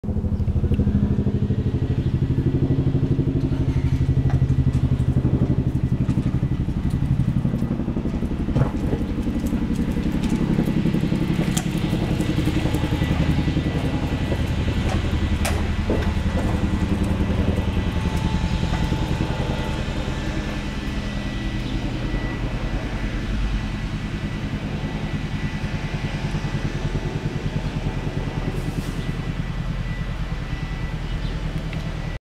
chinook helicopter fly by
Not a great video, but I'm always super excited to see these things in the air. They're so loud and have so much power.